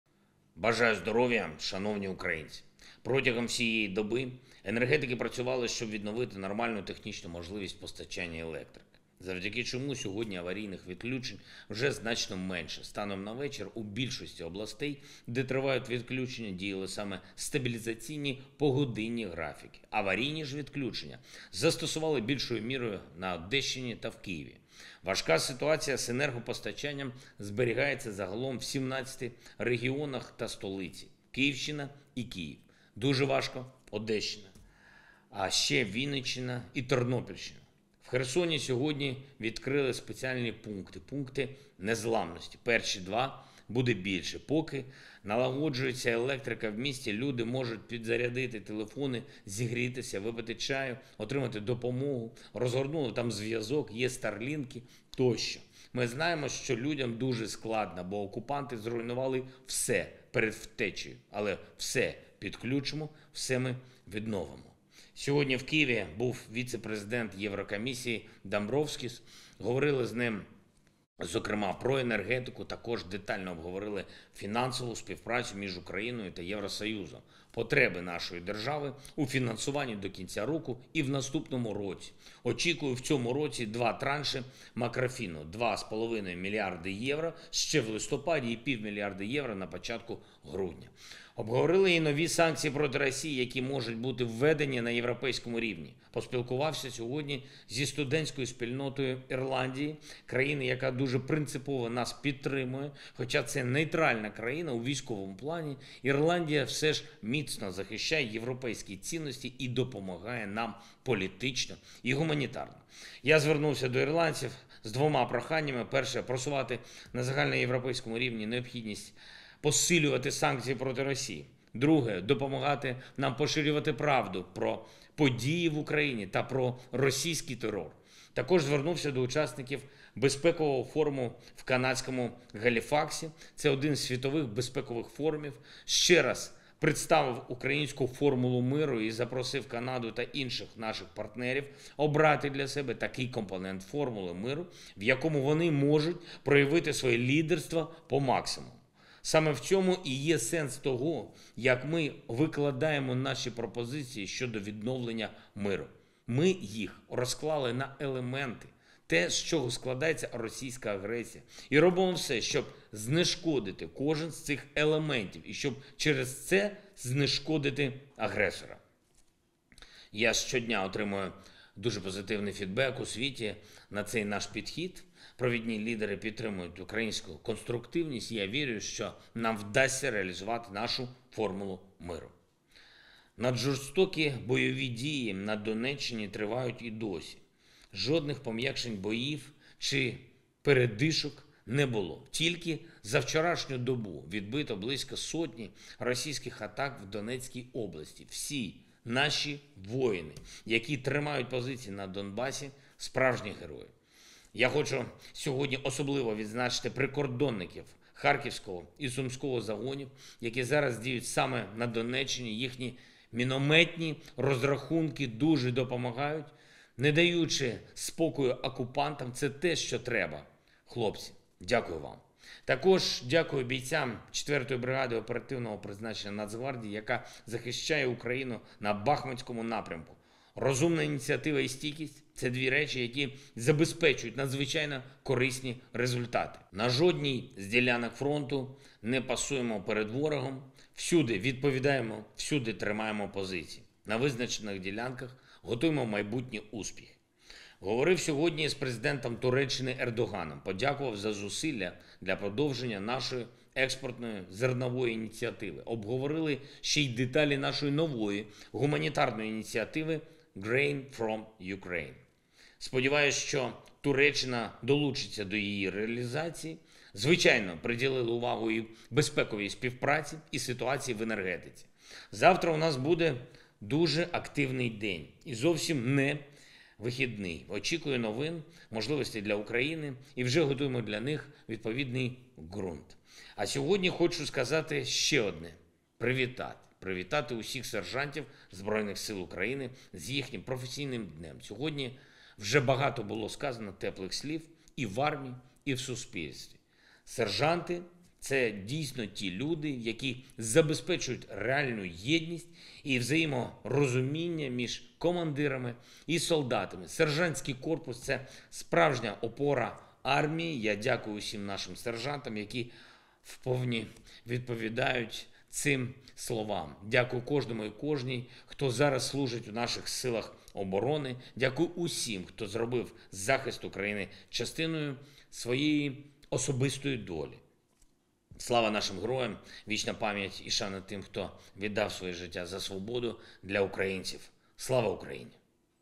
Звернення Президента наприкінці 268-го дня повномасштабної війни
«Наджорстокі бойові дії на Донеччині тривають і досі, жодних помʼякшень боїв чи передишок не було. Тільки за вчорашню добу відбито близько сотні російських атак у Донецькій області. Усі наші воїни, які тримають позиції на Донбасі, – справжні герої.

Хочу сьогодні особливо відзначити прикордонників Харківського й Сумського загонів, які зараз діють саме на Донеччині. Їхні мінометні розрахунки дуже допомагають, не даючи спокою окупантам. Це те, що треба, хлопці!

Також дякую бійцям 4-ї бригади оперативного призначення Нацгвардії, яка захищає Україну на Бахмутському напрямку. Розумна ініціатива та стійкість – це дві речі, які забезпечують надзвичайно корисні результати.

На жодній із ділянок фронту не пасуємо перед ворогом. Усюди відповідаємо, усюди тримаємо позиції. На визначених ділянках готуємо майбутні успіхи».